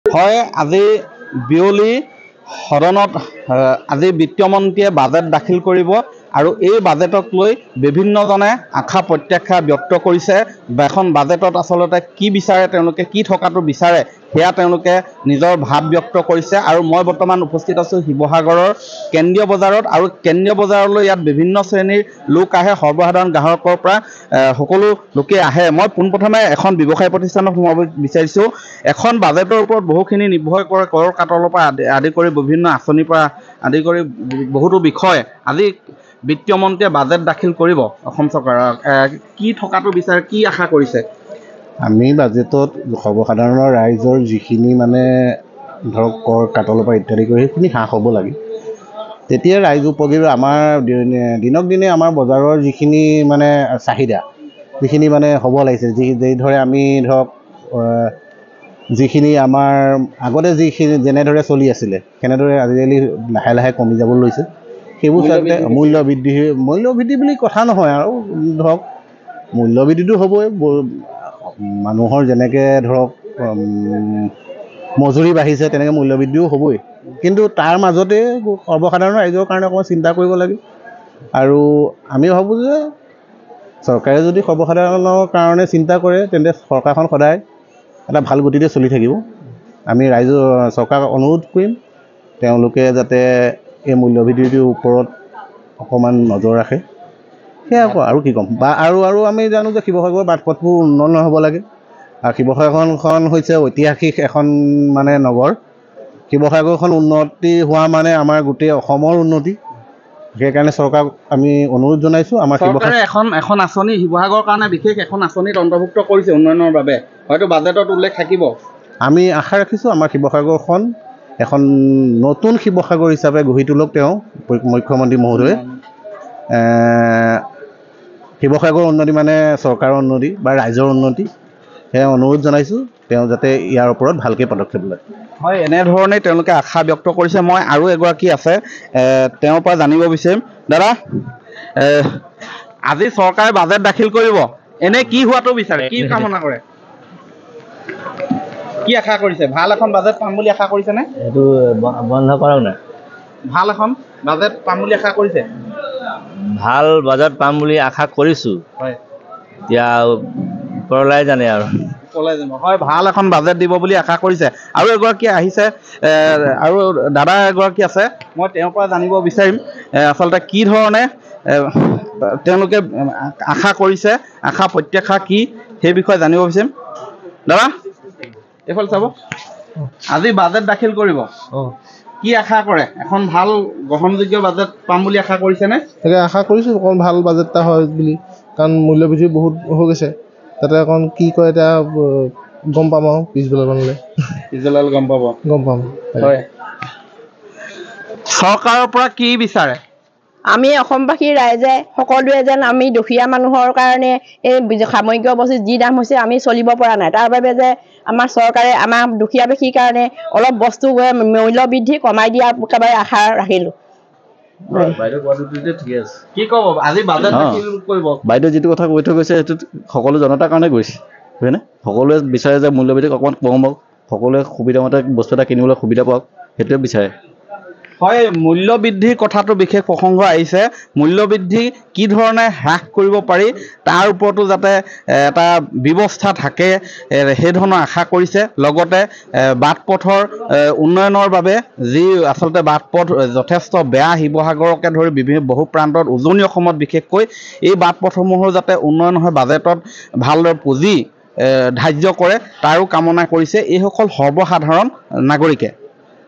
Sivasagar News | STATE BUDGET 2024-25 : বিত্তীয় মন্ত্ৰী অজন্তা নেওগে দাখিল কৰিব বাজেট | n18v
Sivasagar News | STATE BUDGET 2024-25 | আজি বিত্তীয় মন্ত্ৰী অজন্তা নেওগে বিধানসভাত দাখিল কৰিব বাজেট।
বাজেটকলৈ আশাবাদী শিৱসাগৰৰ সৰ্বসাধাৰণ জনতা।